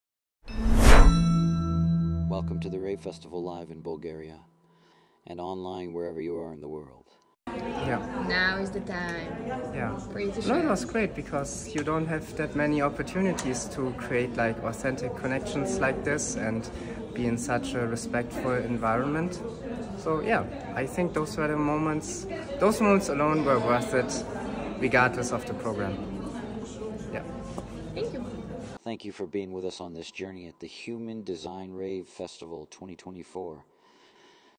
Participants Feedback 10
Participants feedback 10 in the Human Design Rave Festival 2024 in Sofia, Bulgaria